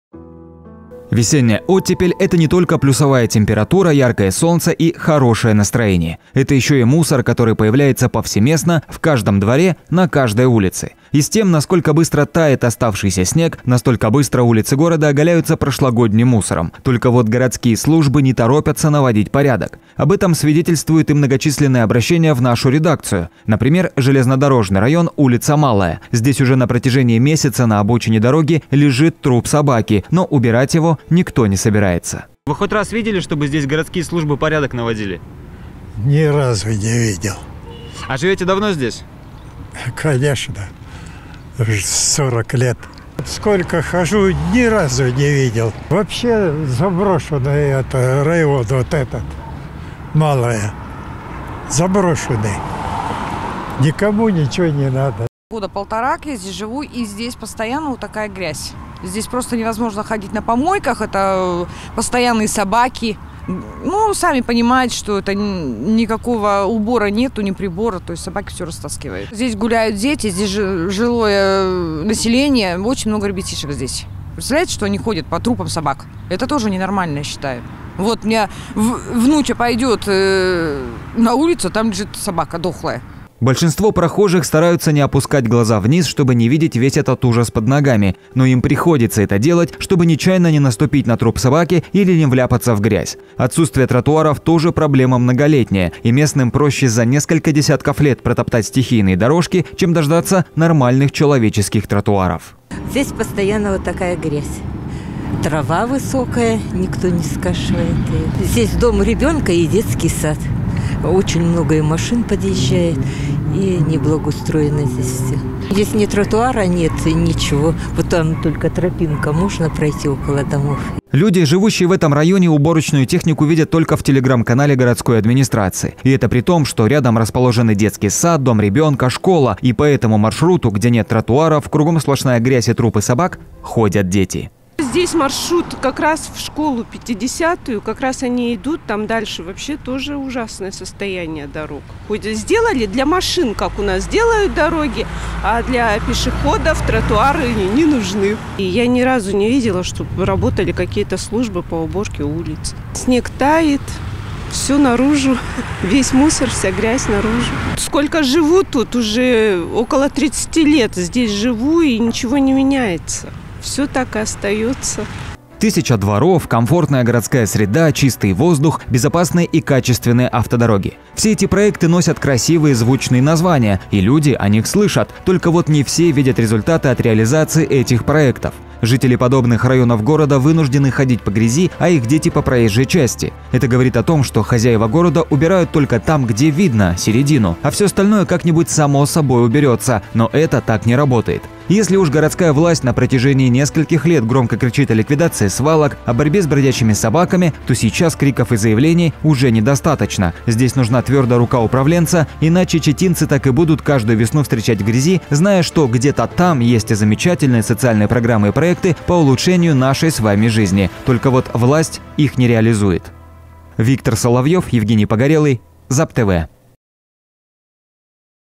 Жители Железнодорожного района: "Наши дети вынуждены ходить по трупам собак"
Насколько эффективно городскими властями исполняются социальные программы, направленные на улучшение качества жизни читинцев? Чтобы найти ответ на этот вопрос, достаточно сесть в автомобиль и проехаться по тем районам города, которые скрыты от большинства горожан. Подробности далее в сюжете.